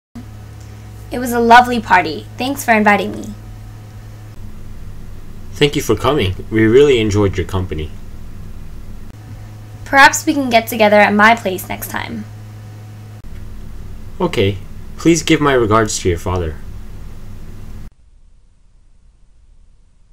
121228 오늘의 대화 총정리
A: It was a lovely party. Thanks for inviting me.
B: Thank you for coming. We really enjoyed your company.
A: Perhaps we can get together at my place next time.
B: Ok. Please give my regards to your father.